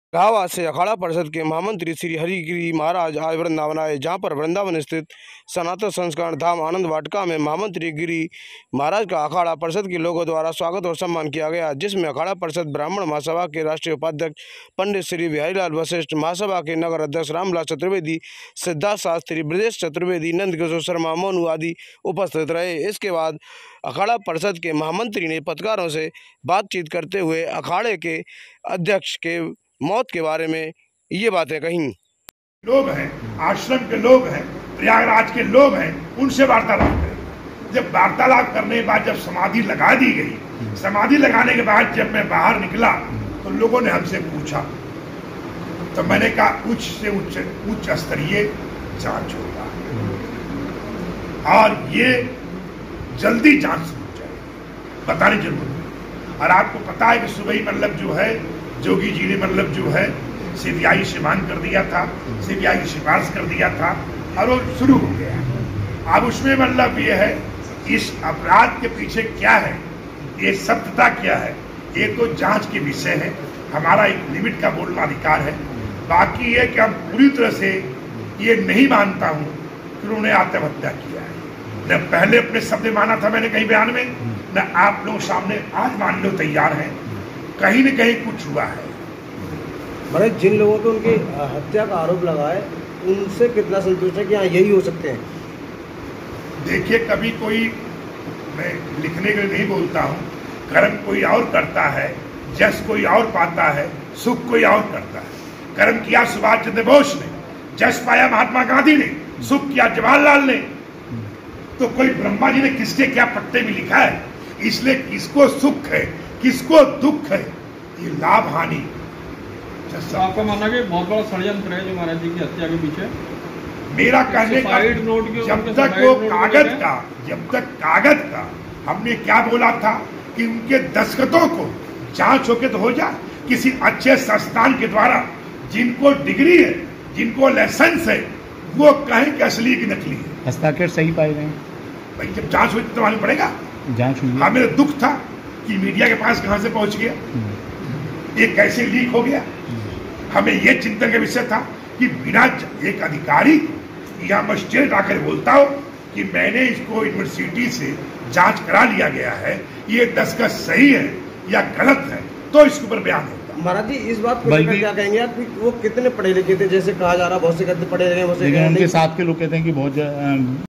अखाड़ा परिषद के महामंत्री ने अध्यक्ष नरेंद्र गिरी महाराज की हत्या पर उठाए सवाल#श्रीहरि गिरी महाराज#
प्रयागराज से अखाड़ा परिषद के महामंत्री श्री हरि गिरि महाराज आज वृंदावन आय जहां पर बृंदावन स्तिथ सनातन संस्कार धाम आनंद वाटिका में महामंत्री हरि गिरि महाराज का अखाड़ा परिषद के लोगों द्वारा स्वागत और सम्मान किया गया जिसमें
अखाड़ा परिषद  ब्राह्मण महासभा के राष्ट्रीय उपाध्यक्ष पंडित श्री बिहारीलाल वशिष्ठ महासभा के नगर अध्यक्ष रामविलास चतुर्वेदी सिद्धार्थ शास्त्री बृजेश चतुर्वेदी नंदकिशोर शर्मा सोनू शर्मा आदि अनेकों लोग उपस्थित रह जिसके बाद अखाड़ा परिषद के महामंत्री हरि गिरि महाराज द्वारा पत्रकारों से बातचीत में कहा गया कि वह आज वृंदावन आए हैं और यहां पर व्याप्त बंदरों की समस्याओं को लेकर उनका मन काफी व्यथित हुआ है  उनका कहना था कि हम सरकार से चाहते हैं और यहां के आश्रमों के साथ-साथ अन्य लोगों से भी कि रोजाना के भंडारे में कुछ अंश बंदरों के लिए भी निकाला जाए ताकि वह लोग यहां आने वाले भक्तों और श्रद्धालुओं के साथ-साथ स्थानीय लोगों के ऊपर हमला ना करें और उनके चश्मे बैग न छीने वहीं उन्होंने यमुना प्रदूषण को लेकर के भी कहा की सरकारों को चाहिए कि वह वृंदावन में बहने वाली मां यमुना को शुद्ध और प्रदूषण मुक्त करें जिस तरह से सरकार द्वारा नमामि गंगे योजना बनाई गई और जिस में मां गंगा को शामिल किया गया मगर यमुना मैया को उन्होंने मां गंगा के बतौर नहीं माना है इसी के साथ साथ जब उनसे मठ के महंत और अखाड़ा परिषद के अध्यक्ष की हत्या के बारे में सवाल किया गया तो उन्होंने कहा कि जिस तरह से अभी तक सामने आया है कि जो हत्यारा है वह काफी चतुर और चालाक है जो लोग गिरफ्तार हुए हैं उनके लिए देश की सर्वोच्च एजेंसी जांच कर रही है और अभी कुछ कहना उचित नहीं होगा लेकिन जब अध्यक्ष के लिए उन्होंने कहा कि कमेटी में चुनाव होगा और चुनाव के बाद जिसके पक्ष में अधिक वोट होंगे उसी को मठ का अध्यक्ष चुना जाएगा ।